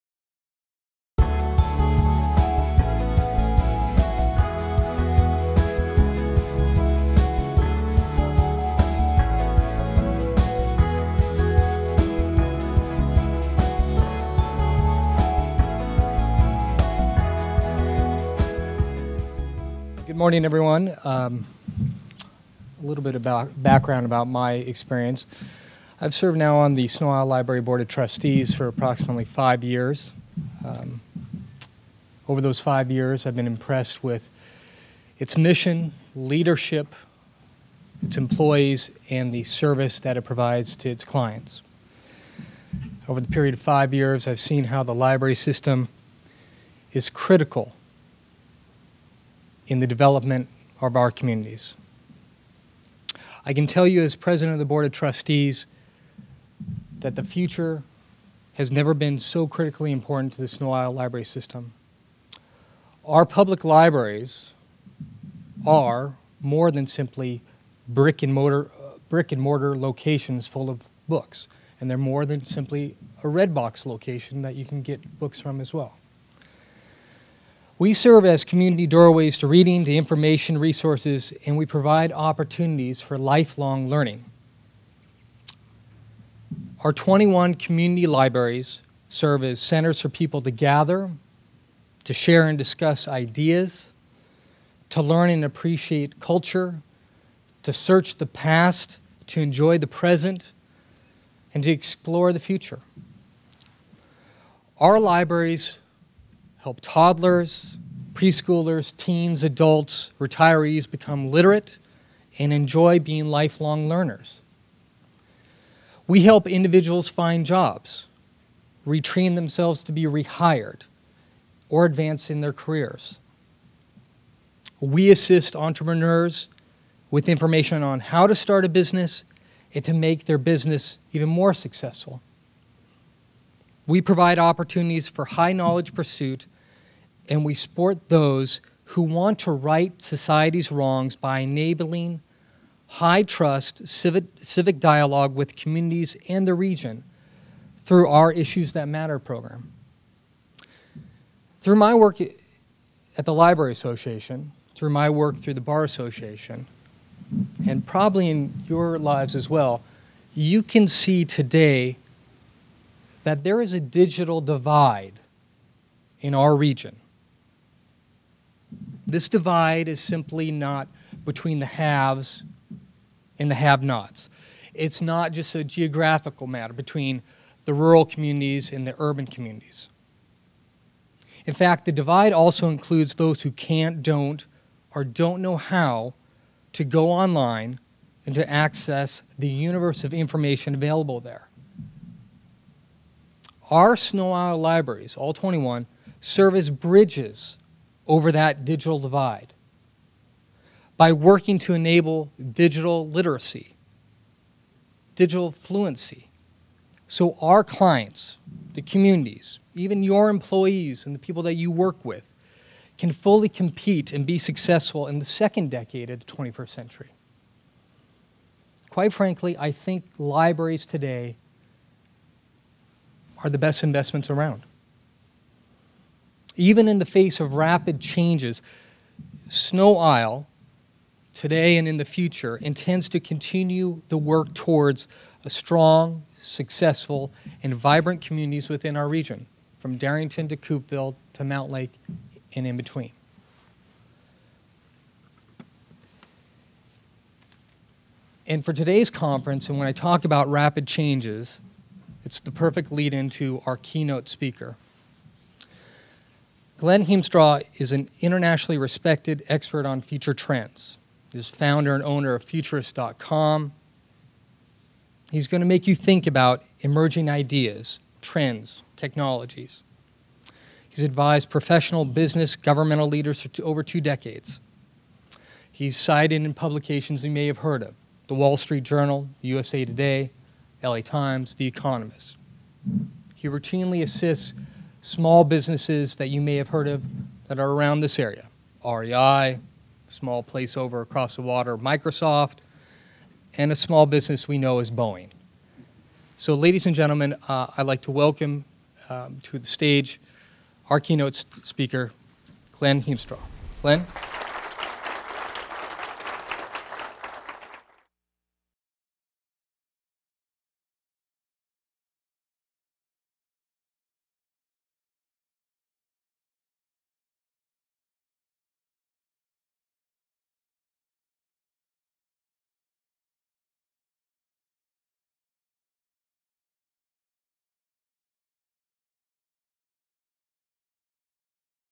Near-Future Visions: Rico Tessandore (part 2 of 4)
"Cultivating the Edges of Risk & Opportunity," March 28, 2013, Introduction of Speaker by Sno-Isle Libraries Board of Trustees President Rico Tessandore. This Sno-Isle Libraries-hosted event gathered business, education, government and library leaders to explore and discuss near-future implications of present-day trends. Keynote speaker, Glen Hiemstra from Futurist.com and a distinguished panel representing City of Edmonds, Edmonds School District, Dillon Works, Inc., Economic Alliance Snohomish County, WorkSource Development Council Snohomish County, and Sno-Isle Libraries.